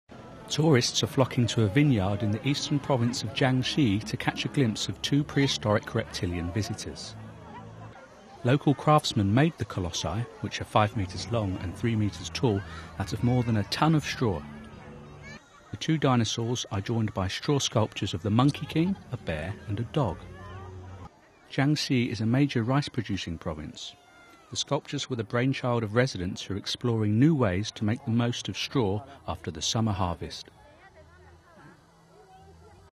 Giant straw dinosaurs stalk visitors to E China vineyard
Tourists are flocking to a vineyard in the eastern province of Jiangxi to catch a glimpse of two prehistoric reptilian visitors.